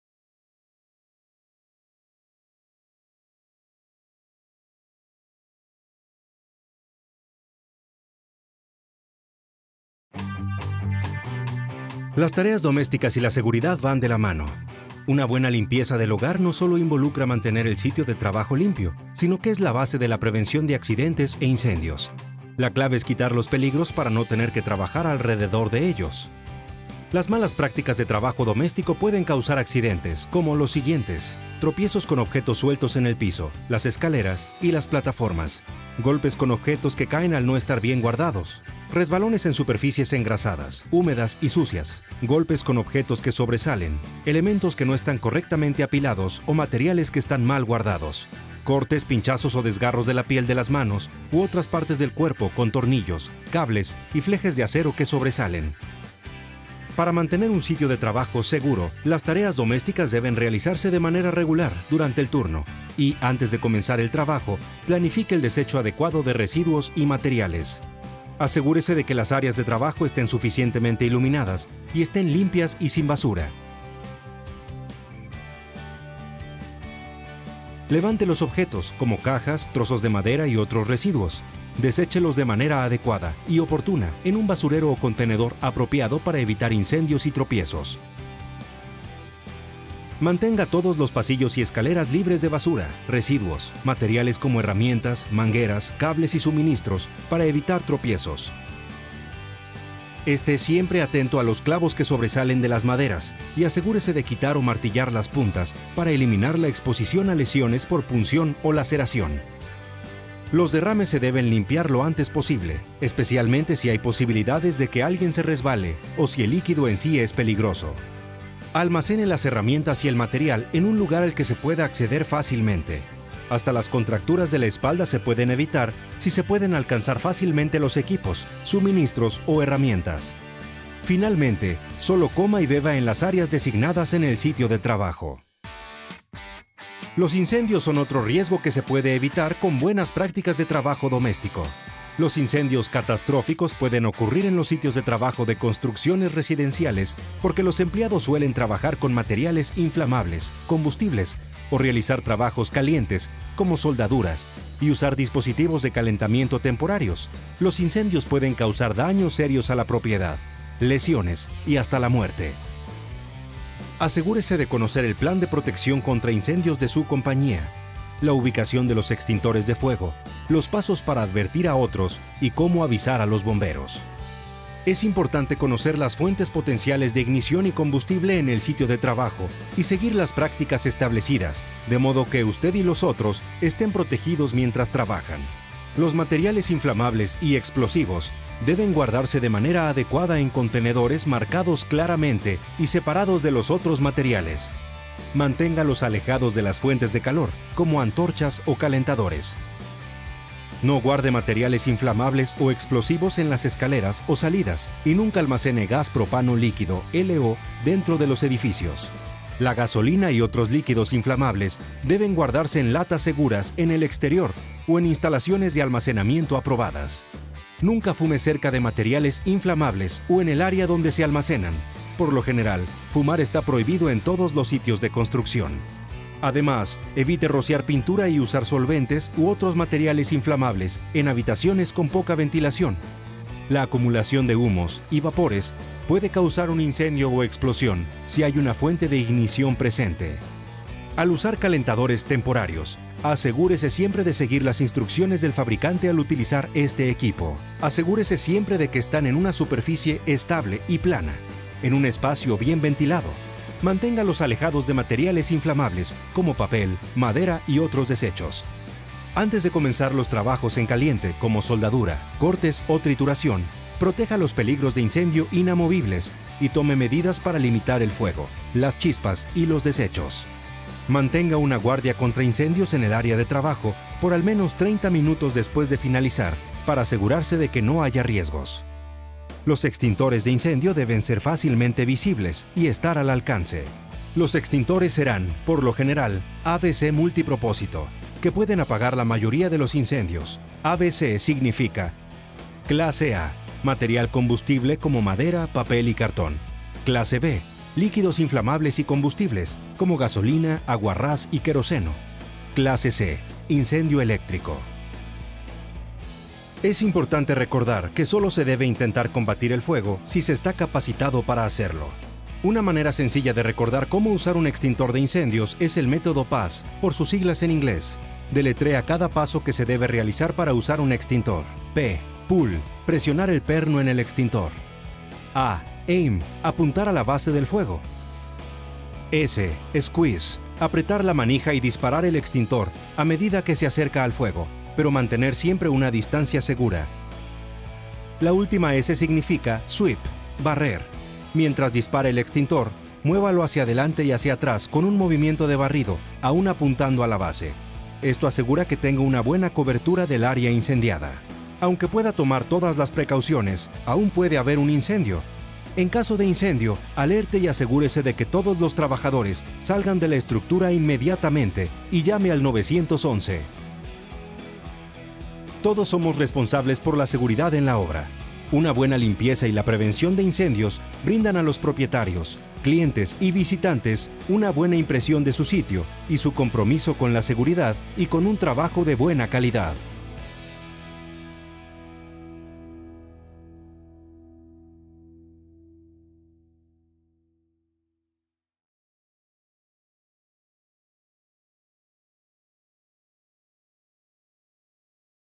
Tareas Domésticas y Protección Contra Incendios (Housekeeping and Fire Protection in Spanish)
This video focuses on job-site housekeeping  and demonstrates practical ways to maintain clean job-sites.  It also discusses ways you can work to prevent fires on your job-sites.